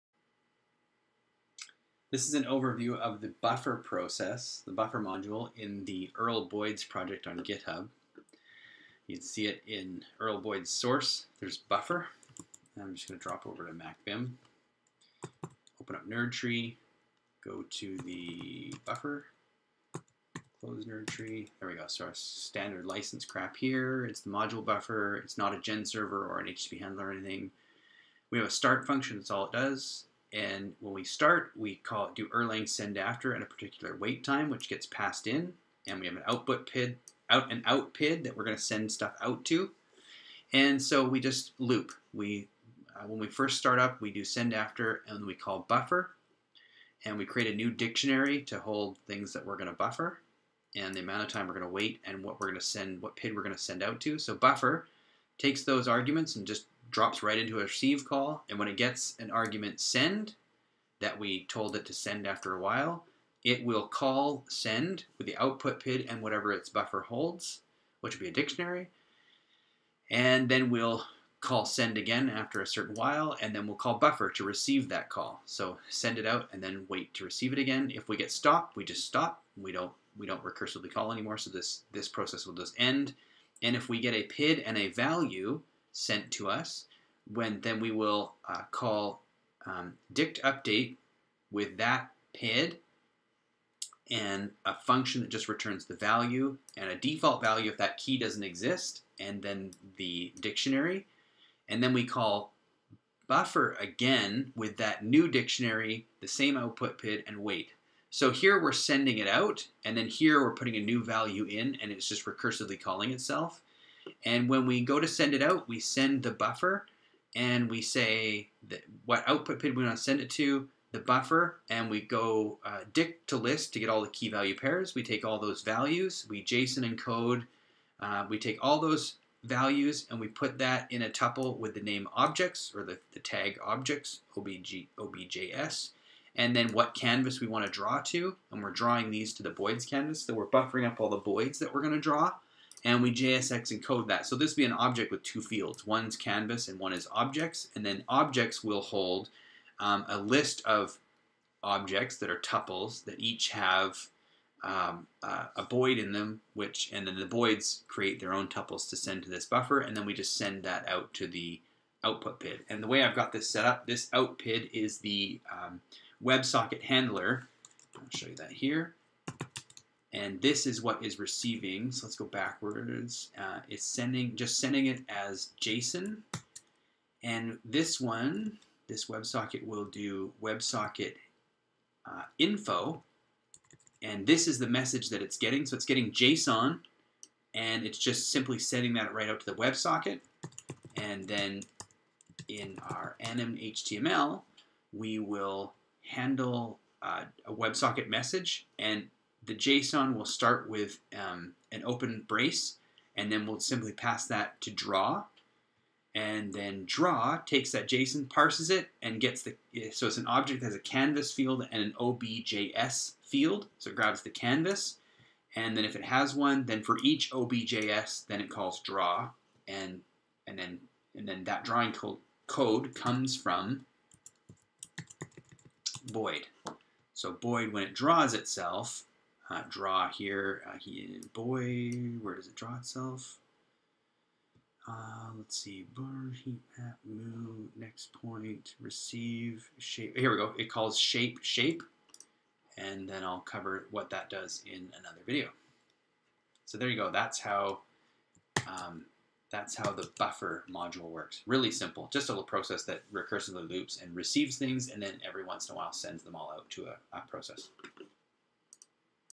erl_boids buffer module (src/buffer.erl)
The buffer gathers up all new drawing objects from autonomous drawing processes and then periodically sends them out (in this case to the websocket handler)